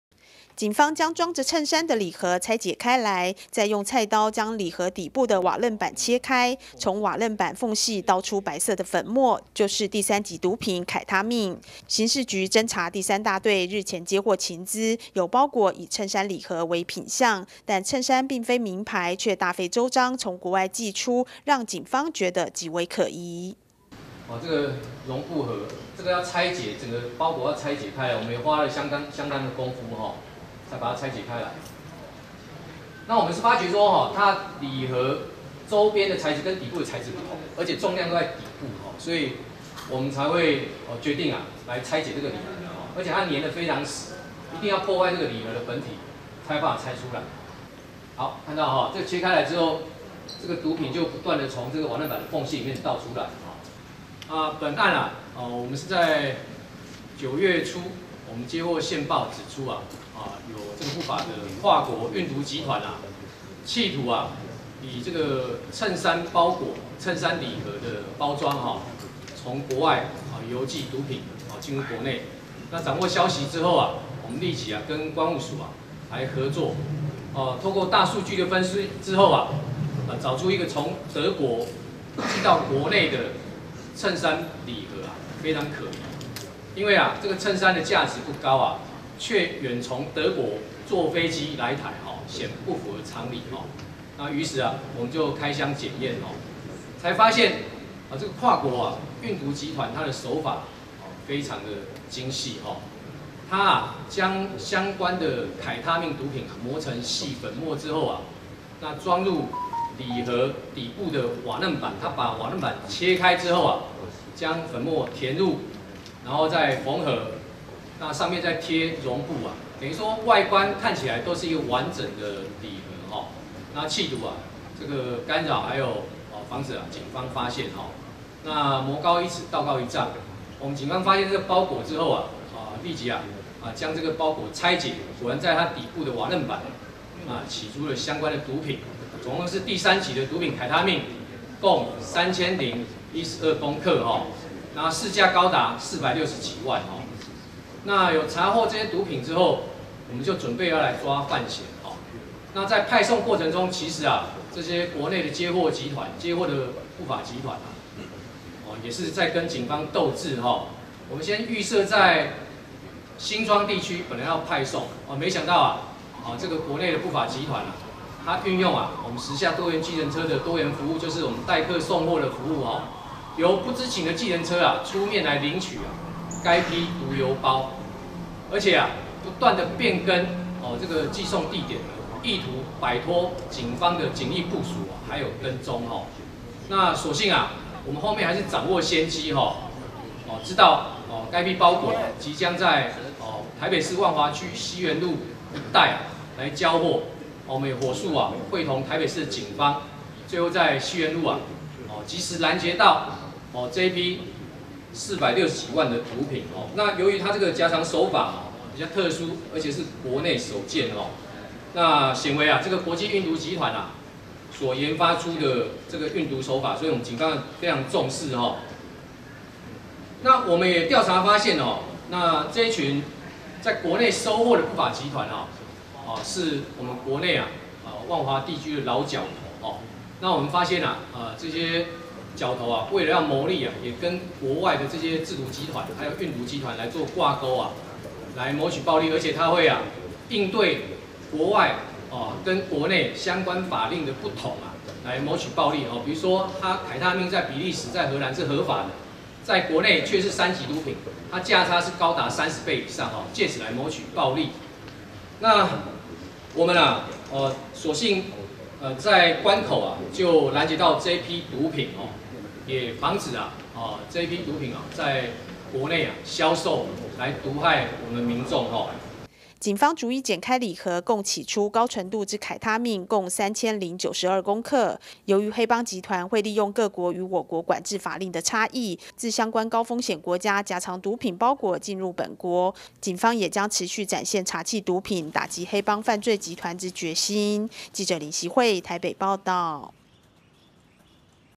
刑事局 毒品|襯衫禮盒夾藏毒品 警破運毒新招
刑事警察局日前接獲情資，有包裹以「襯衫禮盒」為品項，但襯衫並非名牌，卻大費周章從國外寄出，極為可疑；拆開禮盒發現，裏頭居然夾藏第三級毒品「愷他命」。